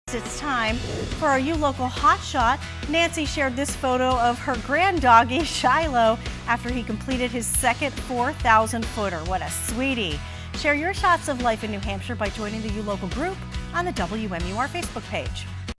Beagle completes second 4,000-footer in New Hampshire
Shiloh completed his second 4,000-footer hike in the White Mountains!